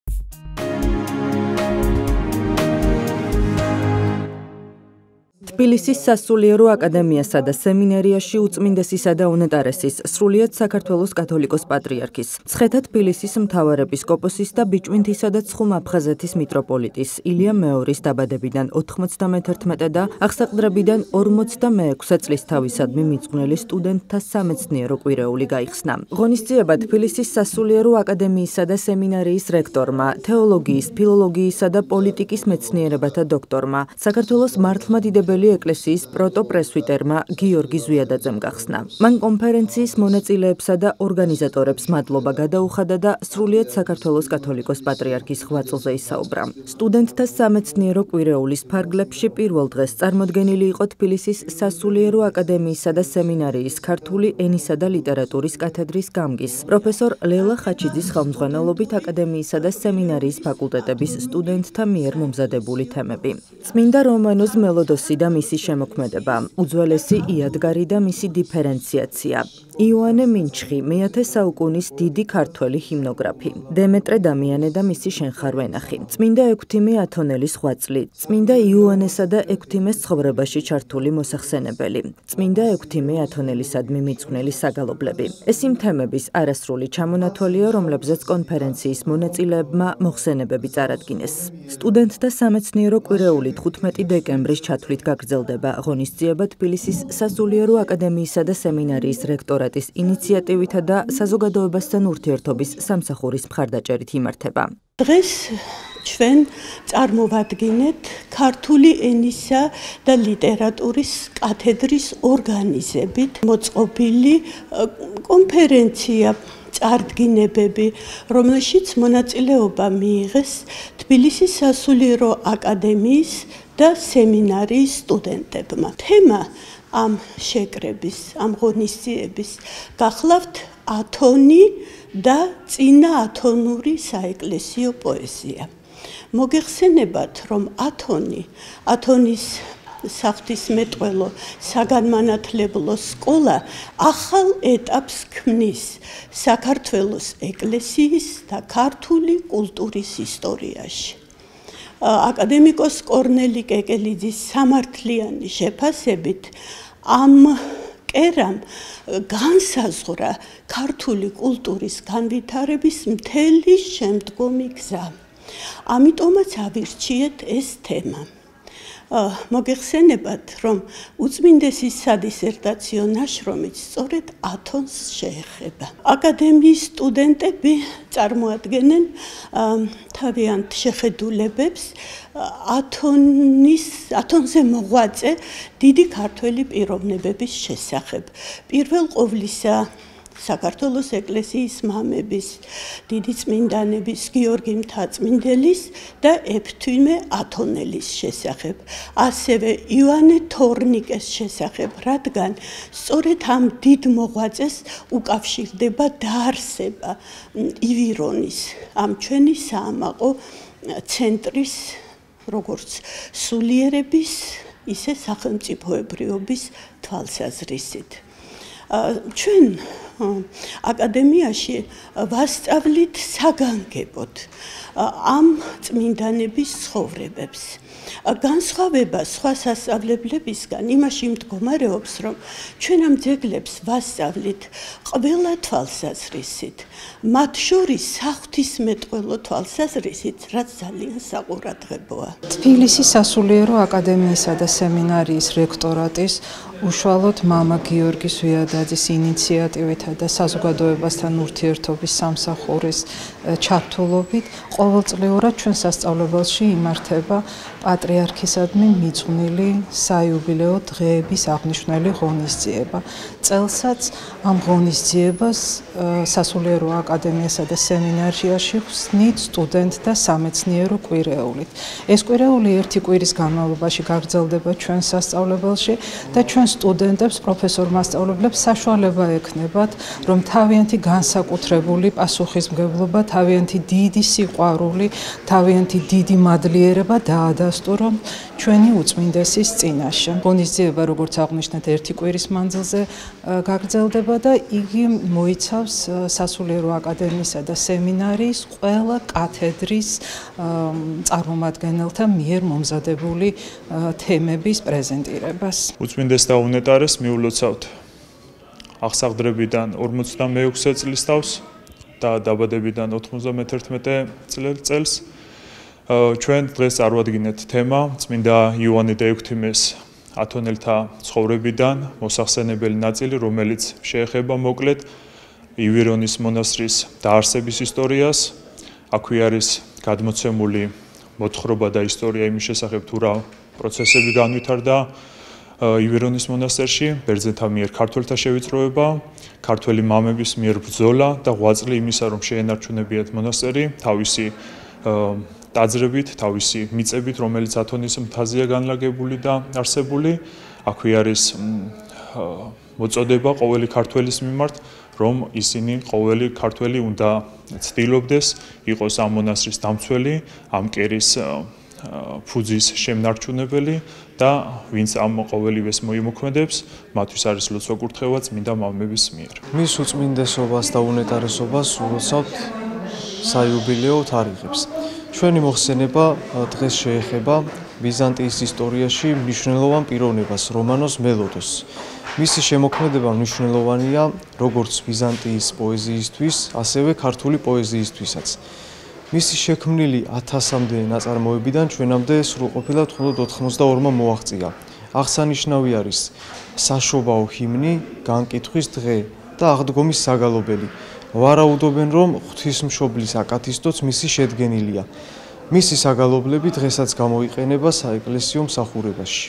თბილისის სასულიერო აკადემიასა და სემინარიაში სამეცნიერო კვირეული გაიხსნა
თბილისის სასულიერო აკადემიასა და სემინარიაში სრულიად საქართველოს კათოლიკოს-პატრიარქის, ილია მეორის  დაბადებიდან 91-ე და აღსაყდრებიდან 46-ე წლისთავისადმი მიძღვნილი სტუდენტთა სამეცნიერო კვირეული გაიხსნა